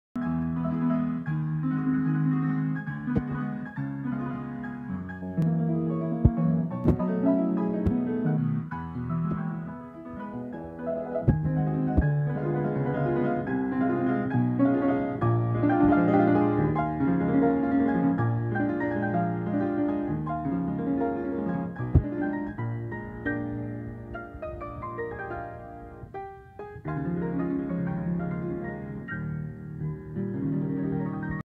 Steinway Grand XP Duo-Art Piano - 00768S
Steinway Grand XP Duo-Art piano with mahogany case. Recent restoration, rebuilt and refinished, plays well with expression, but needs to be tuned. Width: 153 cm. Depth: 190 cm. Condition: Excellent.